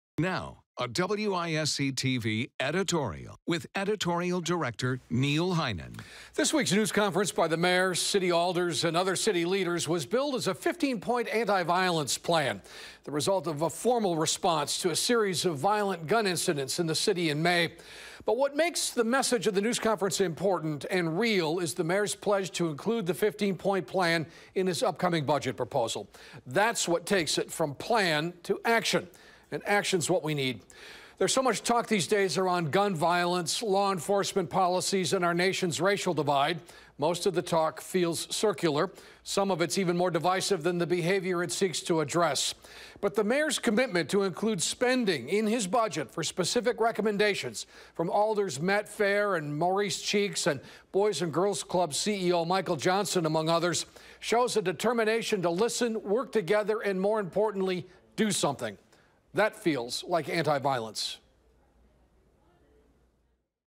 Anti-violence plan deserves spot in Mayor Soglin's proposed budget
This weeks news conference by the mayor, city alders and other city leaders was billed as a 15-point anti-violence plan, the result of formal responses to a series of violent gun incidents in the city in May.